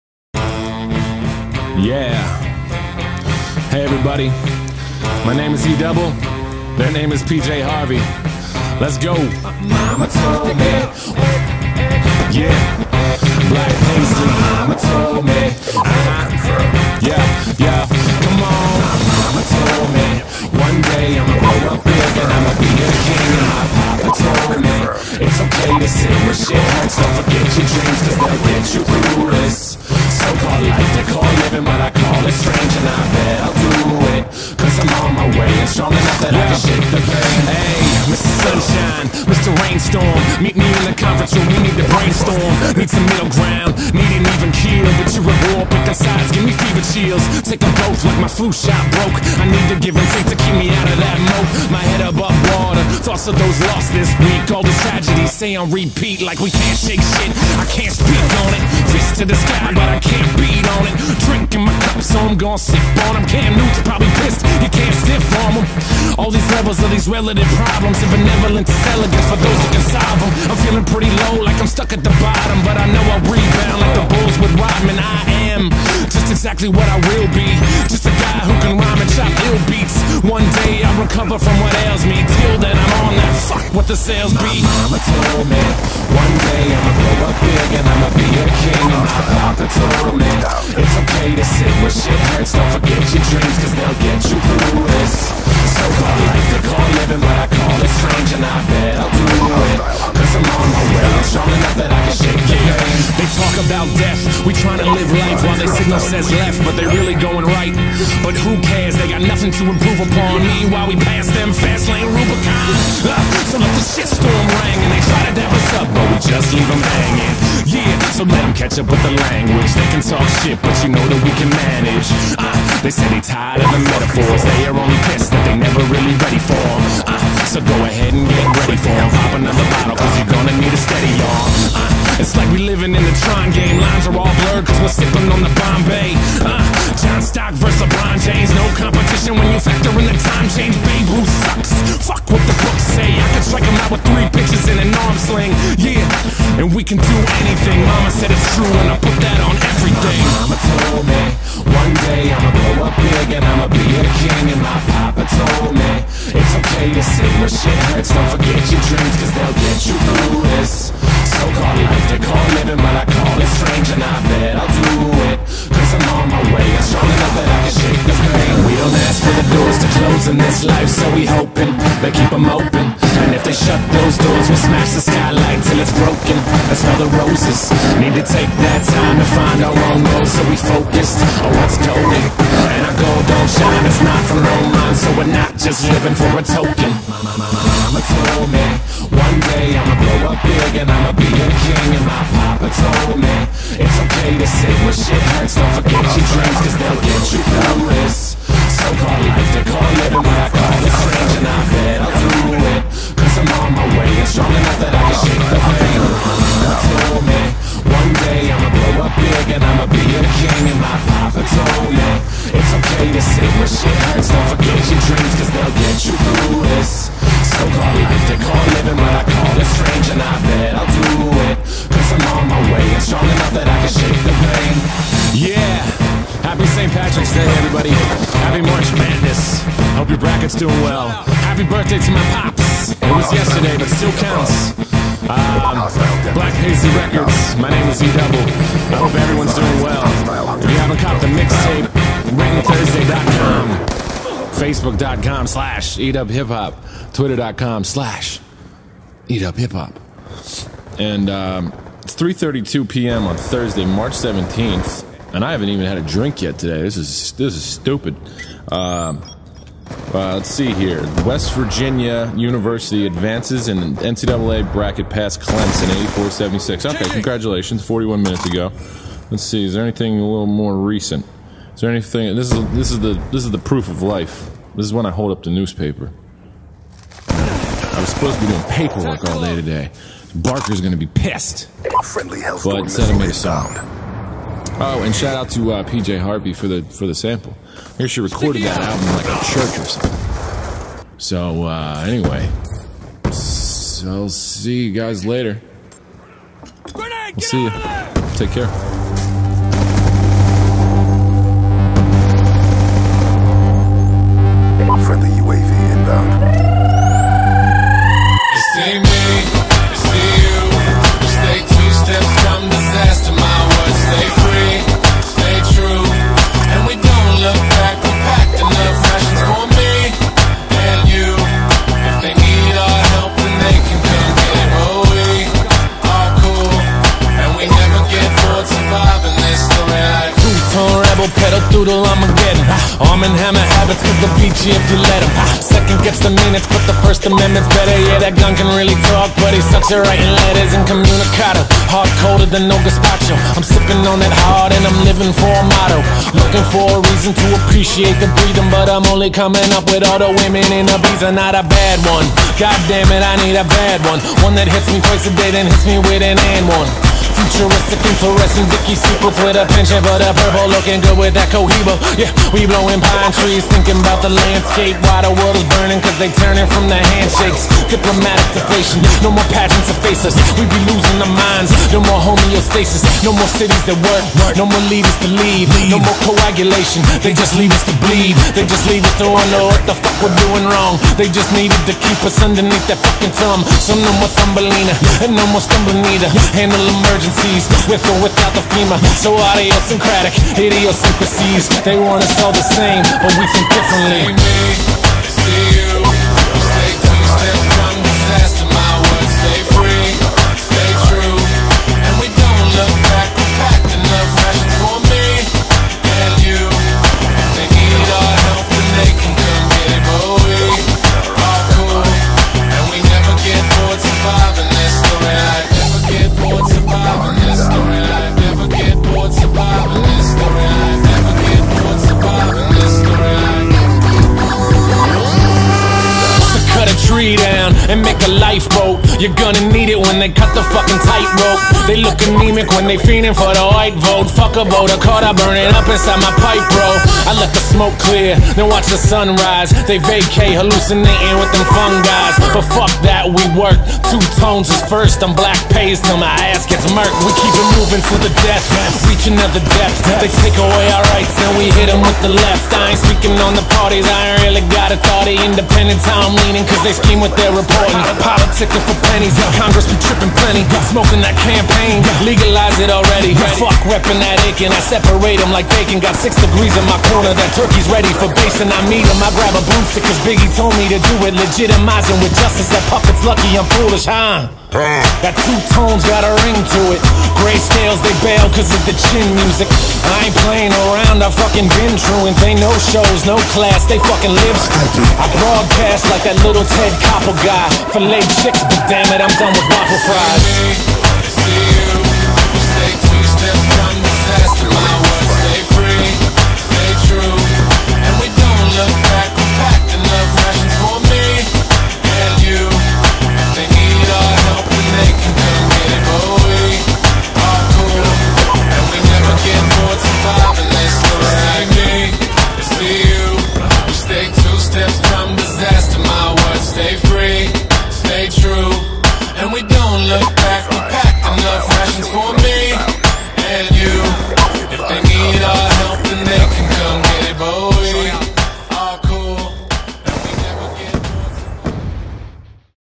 Random Gameplays Episode 2 by xTacosHD
In the second installment of Random Gameplays by xTacosHD. He is using the PDW-57 and goes 22 and 22!